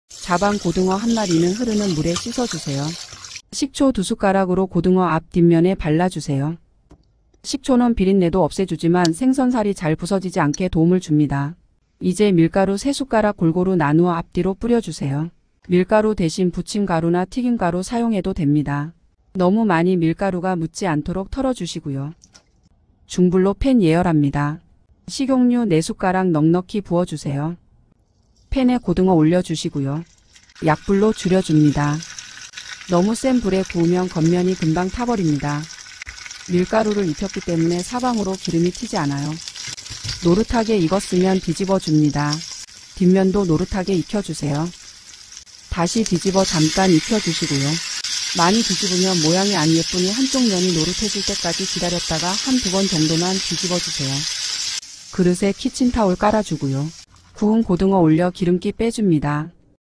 비린내 없고 사방으로 튀지 않는 고등어구이#shorts
고등어구이 쇼츠 영상입니다

요리 초보도 금방 하는 레시피 5편입니다
비린내 싹 잡아 기름 사방으로 튀지 않게 고등어구이 만들어 볼게요
자반 고등어 1마리는 흐르는 물에 씻어 주세요
쌀뜨물에 담가줘도 좋지만 그냥 해도 됩니다
식초 2숟가락으로 고등어 앞,뒷면에 발라 주세요
식초는 비린내도 없애 주지만 생선 살이 잘 부서지지 않게 도움을 줍니다
굽고 나면 식초 냄새는 전혀 나지 않는답니다
이제 밀가루 3숟가락 골고루 나누어 앞뒤로 뿌려 주세요
이 때 체망을 이용하면 골고루 뿌려져서 좋더라구요
밀가루 대신 부침가루나 튀김가루 사용해도 됩니다
너무 많이 밀가루가 묻지 않도록 털어 주시구요
중불로 팬 예열합니다
식용유 4숟가락 넉넉히 부어 주세요
팬에 고등어 올려 주시구요
약불로 줄여 줍니다
너무 센 불에 구우면 겉면이 금방 타버립니다
밀가루를 입혔기 때문에 사방으로 기름이 튀지 않아요
노릇하게 익었으면 뒤집어 줍니다
뒷면도 노릇하게 익혀 주세요
다시 뒤집어 잠깐 익혀 주시구요
많이 뒤집으면 모양이 안 예쁘니 한 쪽 면이 노릇해질 때까지 기다렸다가
한 두 번 정도만 뒤집어 주세요
그릇에 키친타올 깔아 주구요
구운 고등어 올려 기름기 빼줍니다

grilled mackerel